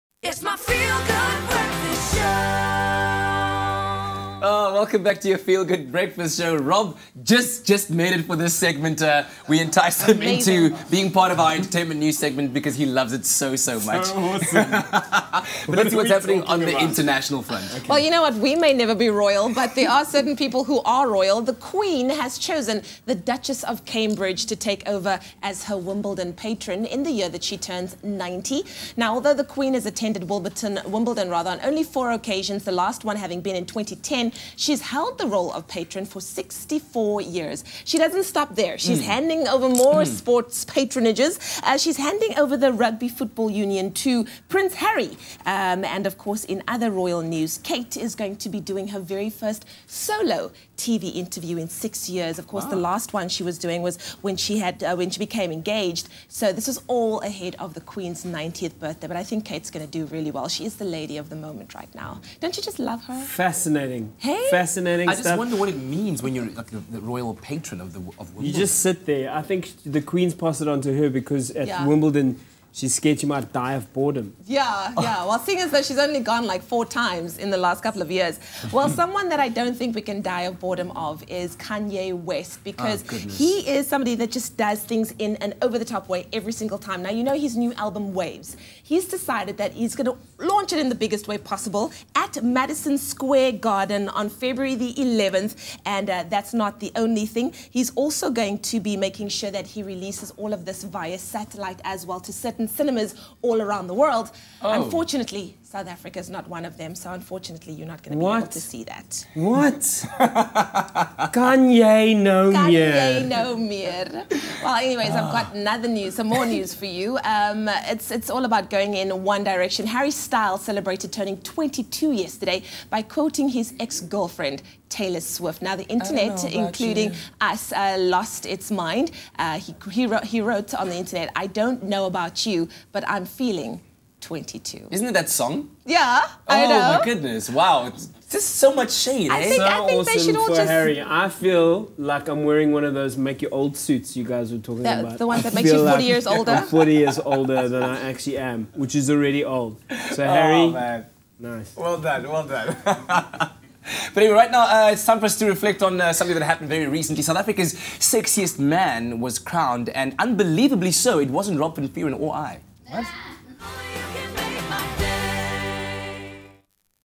Harry Styles quotes Taylor Swift / Kate Middleton Becomes Wimbledon Patron
The Queen has chosen the Duchess of Cambridge take over from her as Wimbledon patron, in the year that she turns 90. Although the Queen has attended Wimbledon on just four occasions, most recently in 2010, she has held the role of patron for 64 years.